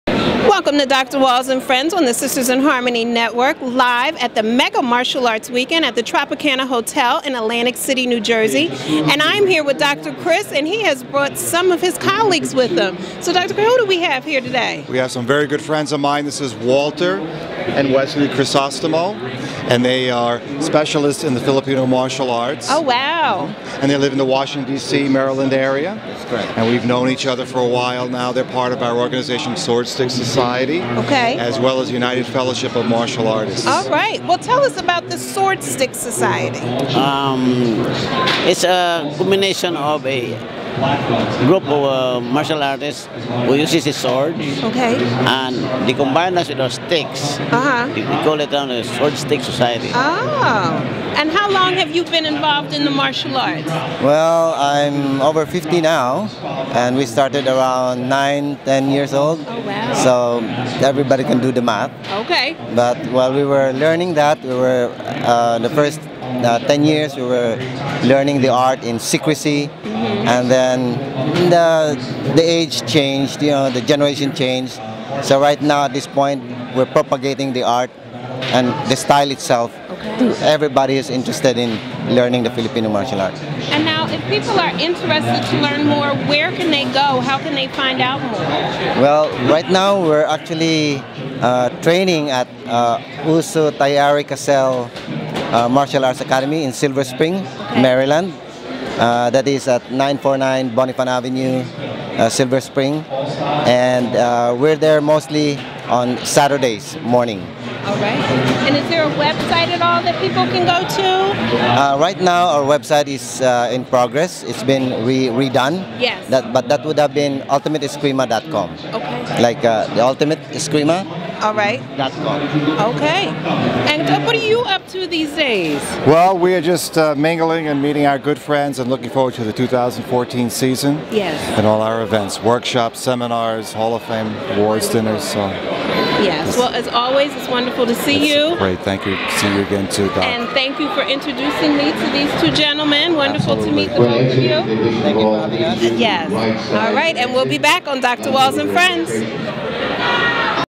DR WALLS SPEAKING DR. CHRIS AND FRIENDS
DR WALLS SPEAKING WITH DR. CHRIS AND SOME OF HIS FRIENDS AT THE MEGA MARTIALS ARTS WEEKEND